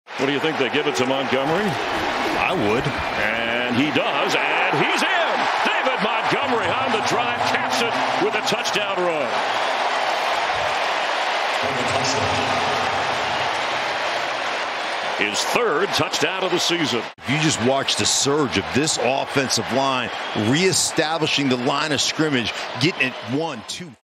David Montgomery rushes up middle for opening score vs. Cardinals
Detroit Lions running back David Montgomery rushes for a 1-yard touchdown against the Arizona Cardinals.